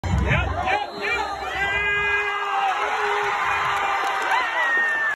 Isaac Smith Hixson High School 2024 Kicker.
Isaac Smith #45 Hixson, with a game winning 41 yd FG with 6 seconds left to give Hixson their FIRST win ever over Signal Mountain